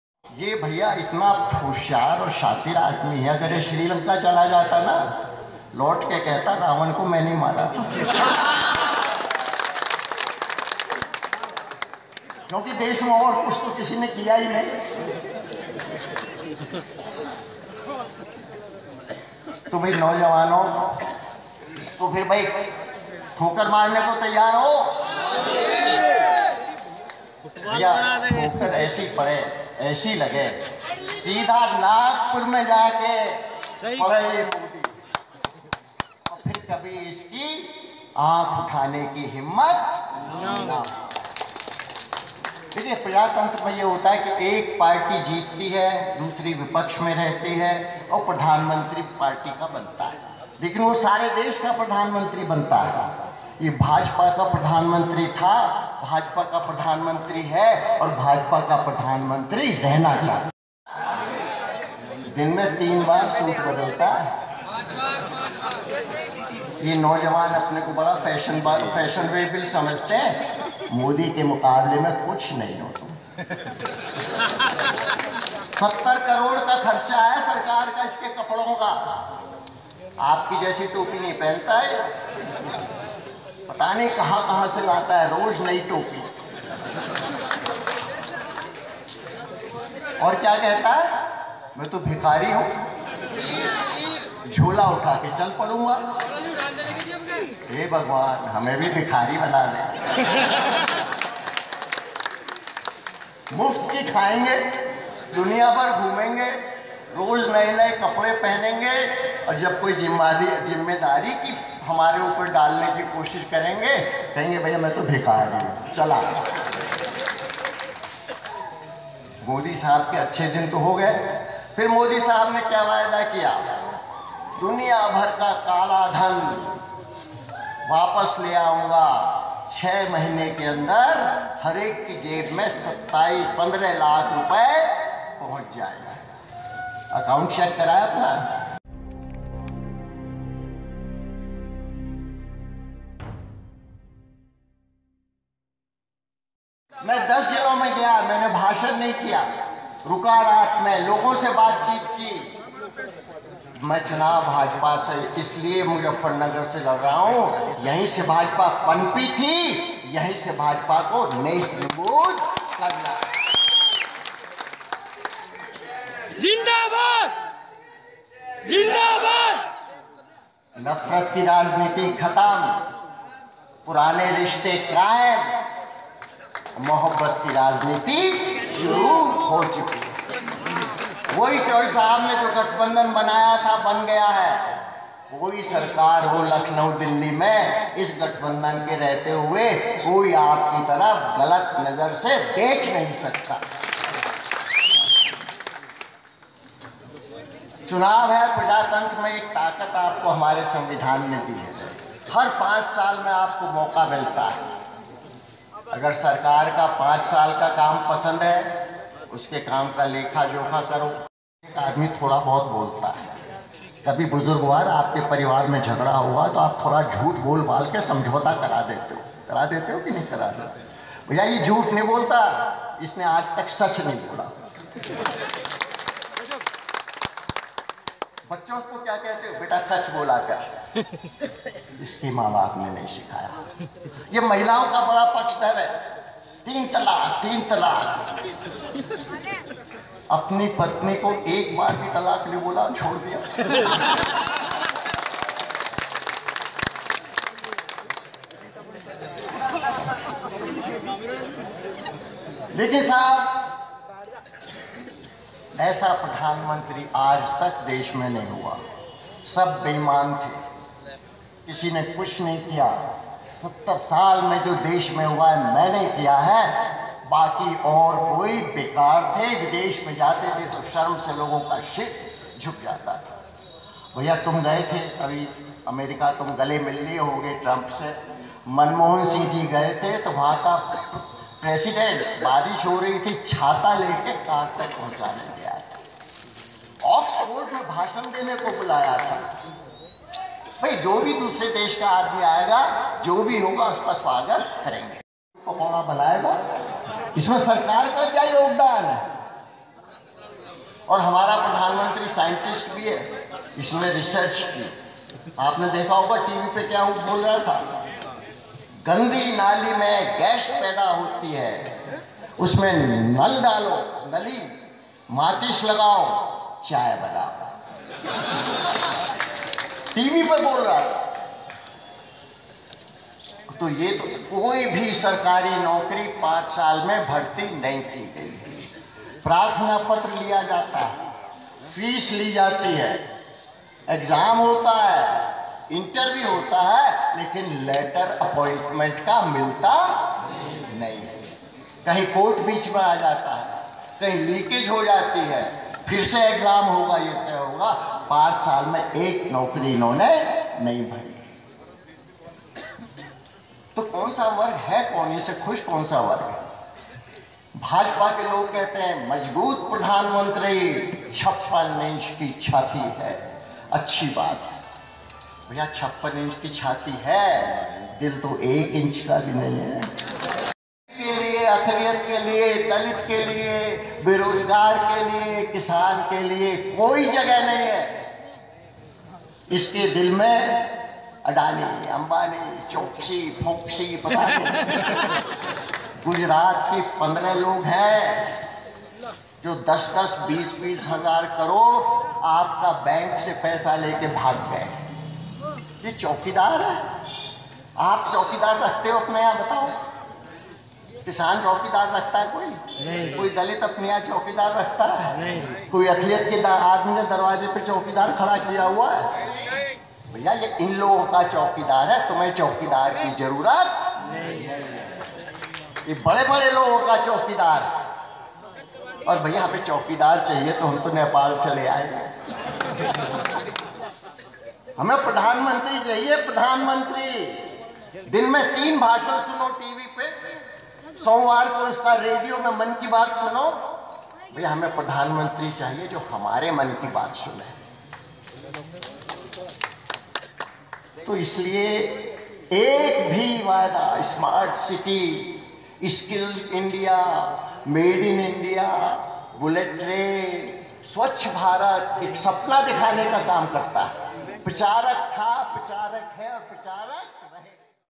अजीत सिंह बोले “अगर पीएम मोदी श्रीलंका जाते तो लौटकर कहते रावण को मैंने ही मारा”
Bagpat news, बागपत। राष्ट्रीय लोकदल (आरएलडी) प्रमुख चौधरी अजित सिंह ने पीएम नरेंद्र मोदी पर ​निशाना साधा। अजित सिंह ने कहा, ''ये महिलाओं का बहुत बड़ा पक्षधर है। तीन तलाक-तीन तलाक कहता है, लेकिन अपनी पत्नी को एक बार भी तलाक नहीं बोला और छोड़ दिया। यही नहीं पीएम मोदी के चौकीदार कैम्पेन पर बोलते हुए कहा ये बड़े लोगों का चौकीदार है। गुजरात के 15 लोग है, जो बैंकों का करोड़ो रुपये लेकर भाग गए, हमें नहीं चाहिए ऐसा चौकीदार और जरूरत होगी तो नेपाल से ले आएंगे।